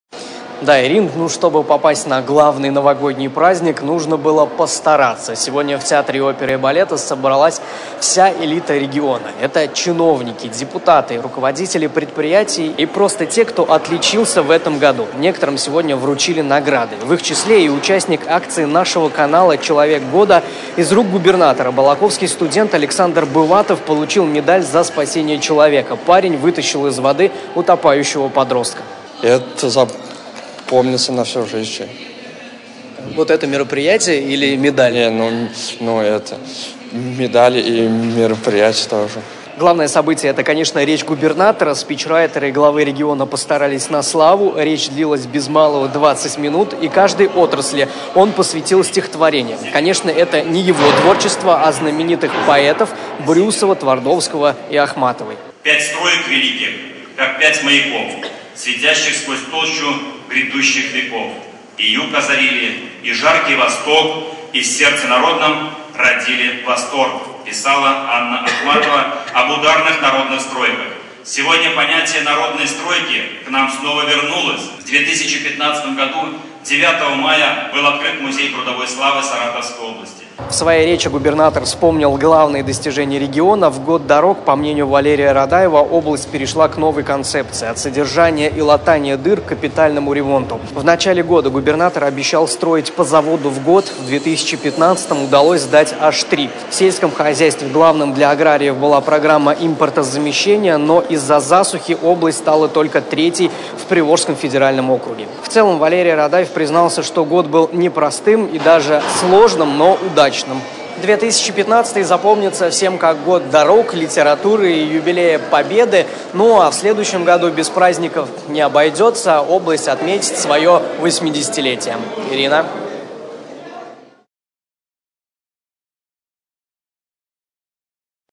Валерий Радаев подвел итоги 2015.
Не простой, но не безрезультатный! Сегодня в театре оперы и балета прошла главная ёлка области. Правда, не для детей. С наступающим Новым годом саратовцев поздравил губернатор. Валерий Радаев подвел итоги две тысячи пятнадцатого и раздал подарки...